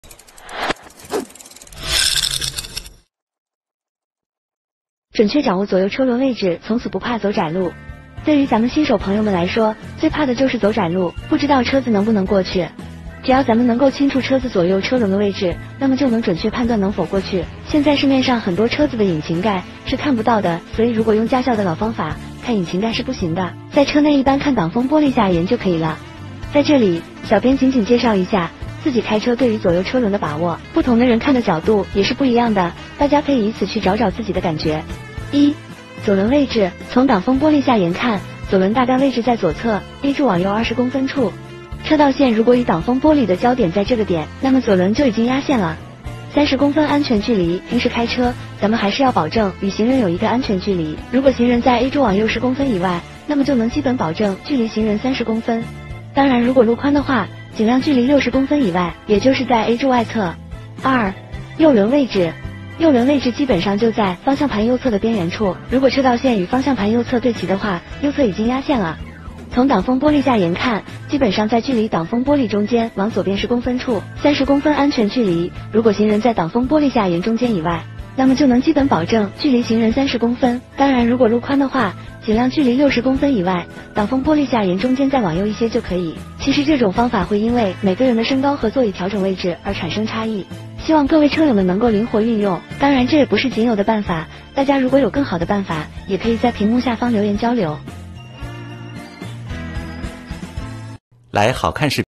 左右车轮位置如何判断？看准这几个点，从此不怕走窄路
左右车轮位置如何判断？看准这几个点，从此不怕走窄路,本视频由COPSPOWER原创提供,4675次播放,好看视频是由百度团队打造的集内涵和颜值于一身的专业短视频聚合平台